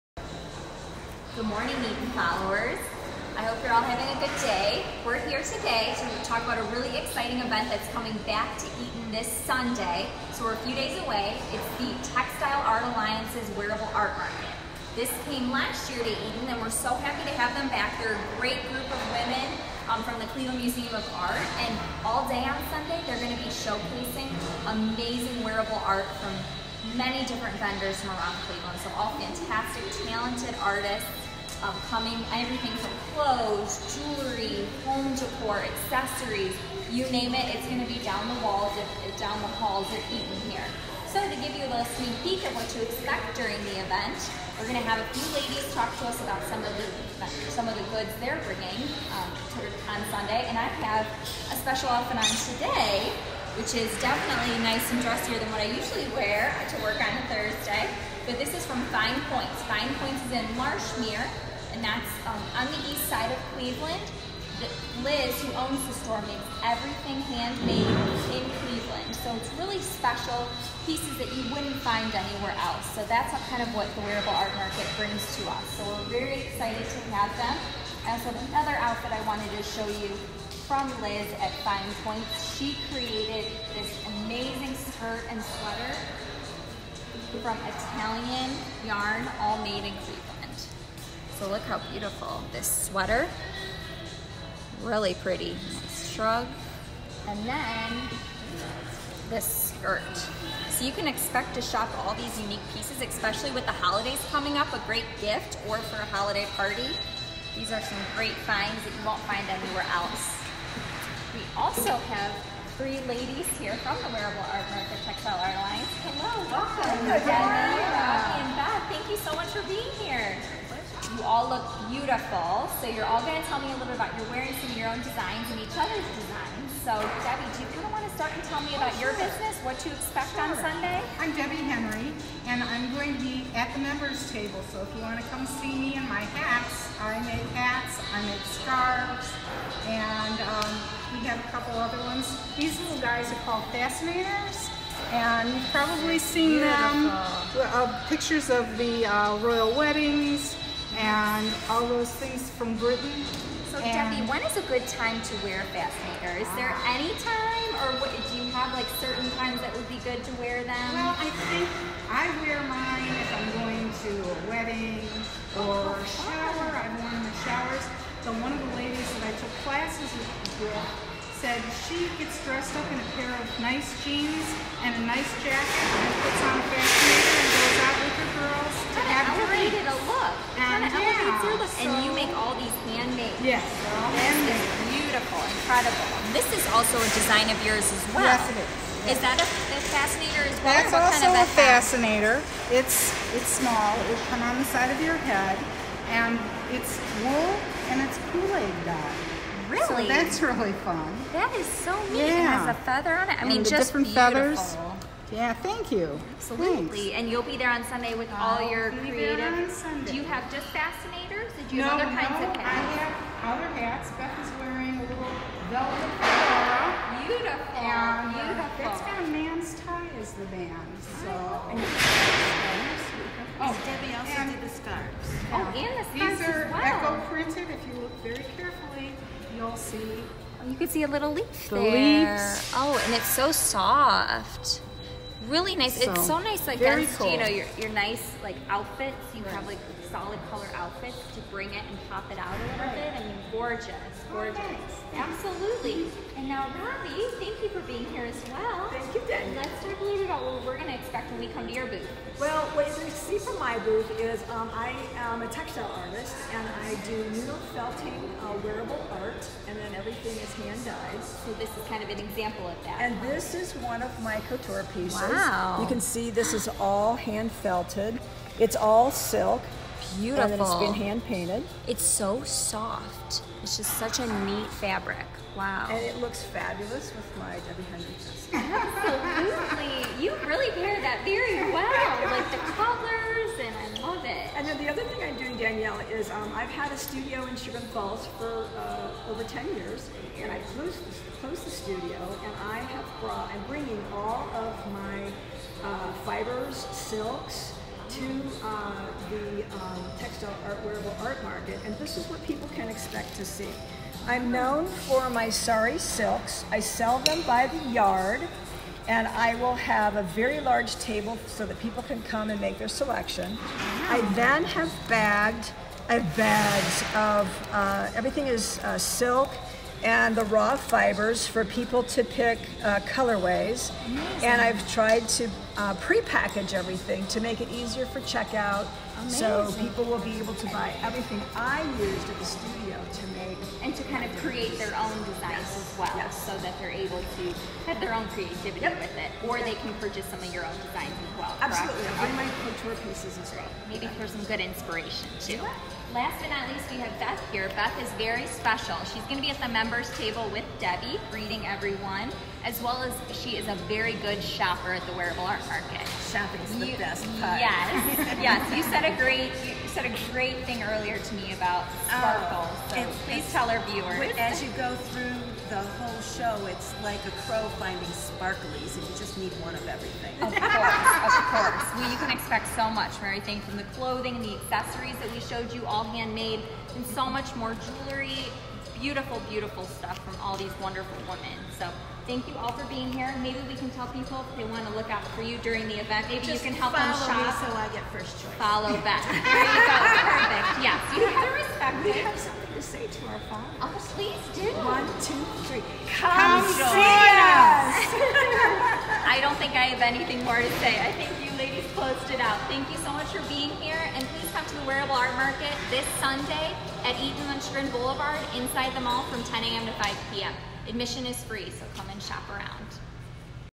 TAA Wearable Art Market
A sneak peek at some of the fantastic handmade pieces coming to Eton Chagrin Blvd for the 2nd Annual Textile Art Alliance Wearable Art Market on October 13th.